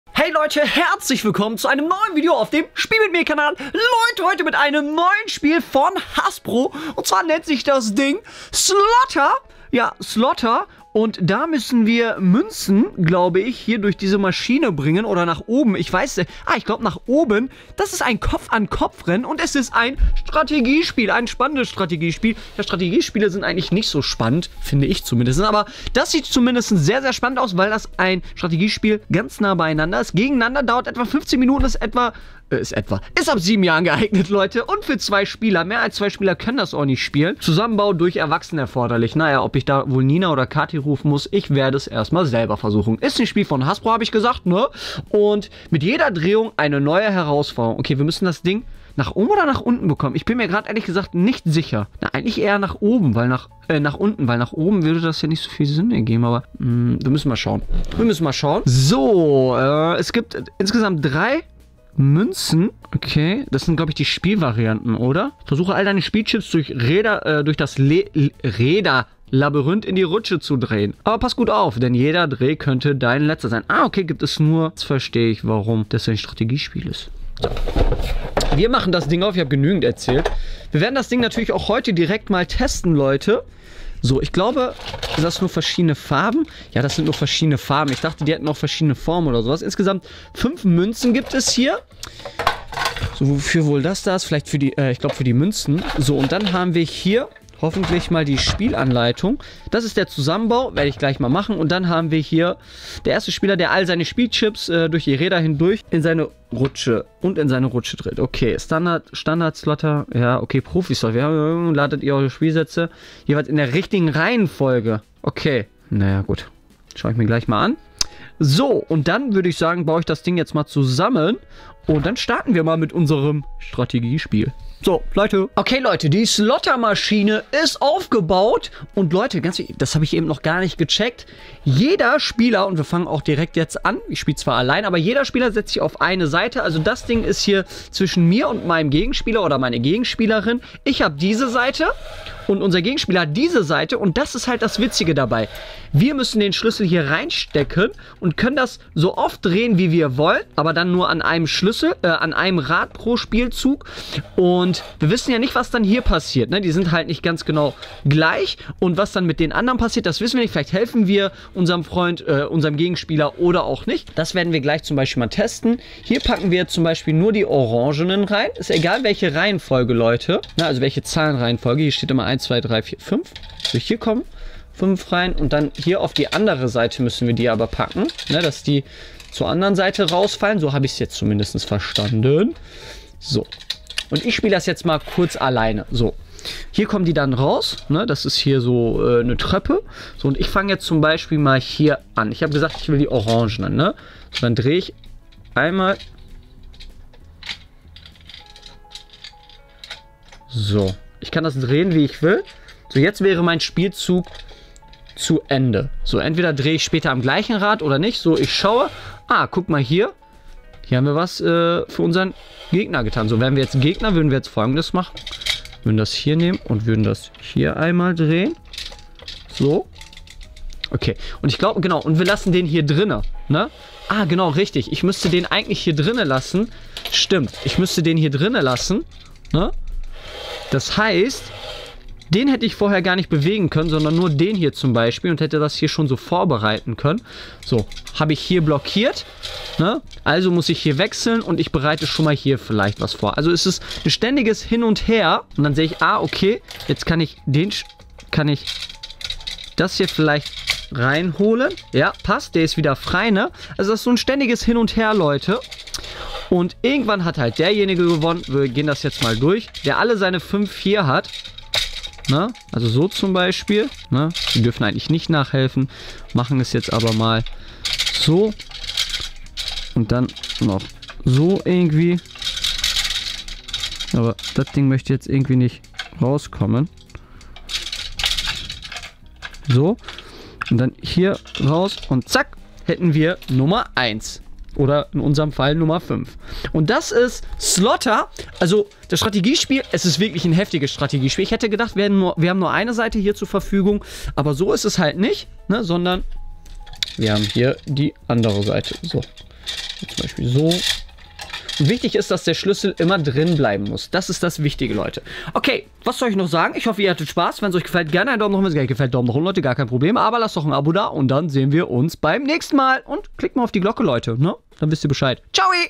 SLOTTER Hasbro Deutsch - WER KNACKT DIESES SPIEL!? Strategiespiele sind nix für Kaan! Spiel mit mir
Kaan testet das neue Strategiespiel SLOTTER von Hasbro!

8228